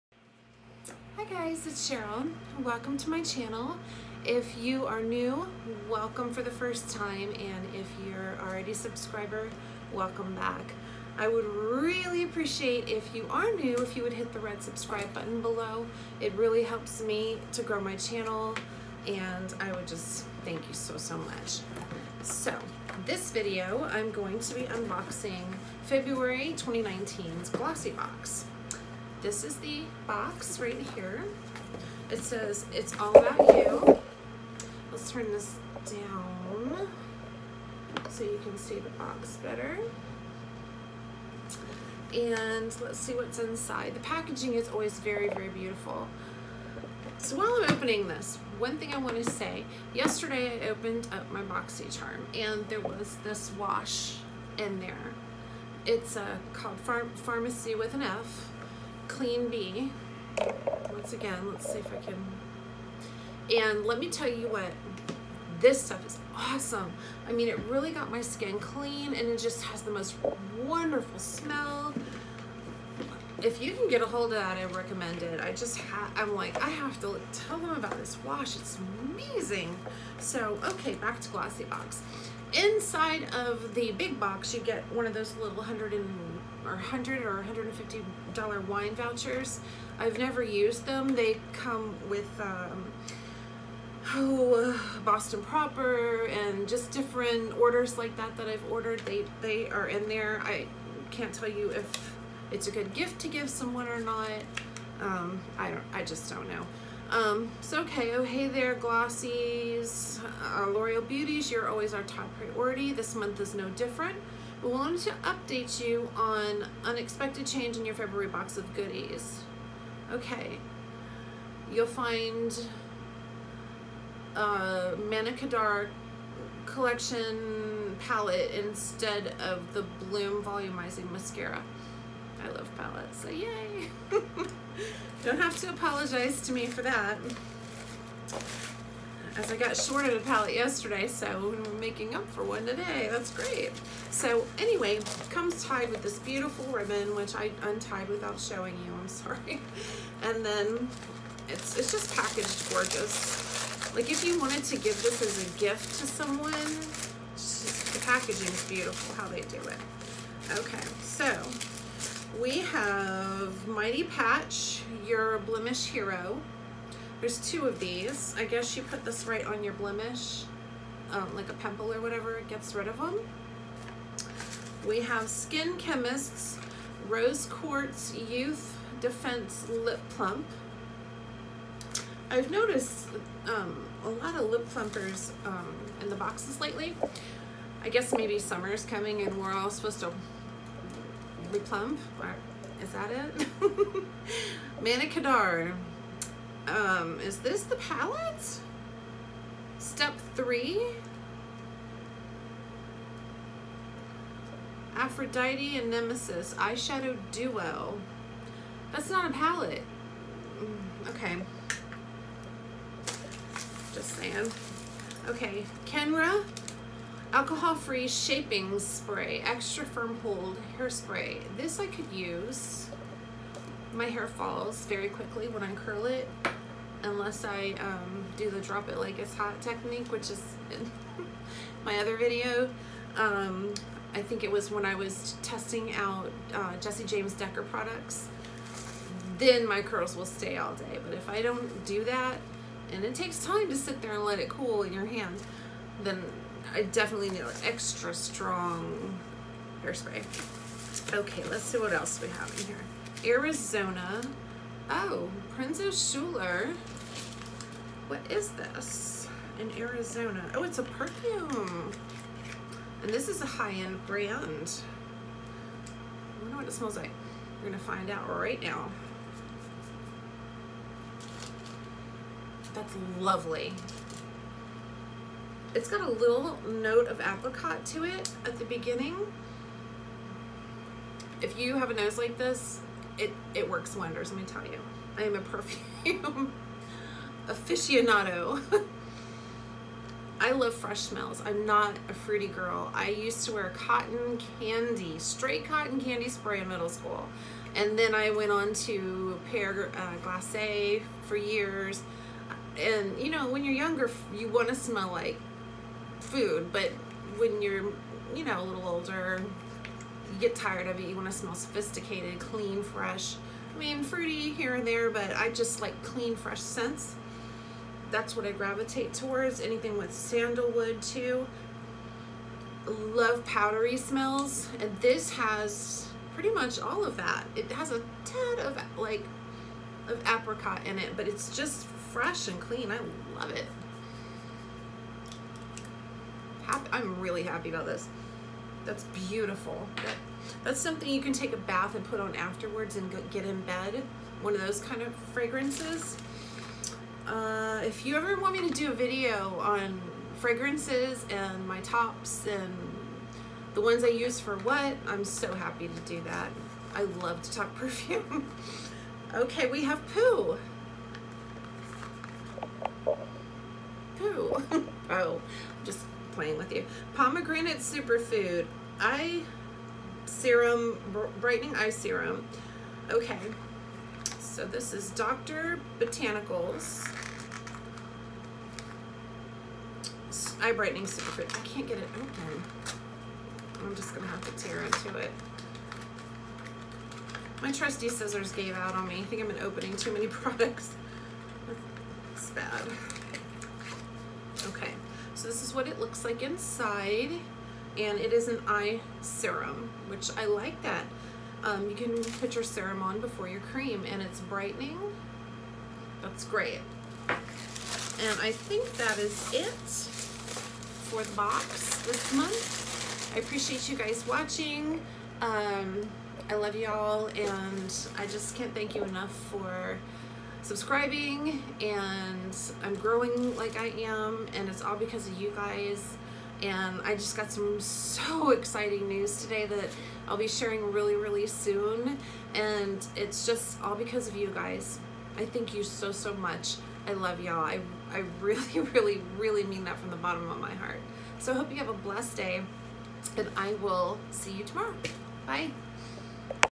Glossybox February 2019
Glossybox February 2019 plus review on farmacy clean 🐝 bee face wash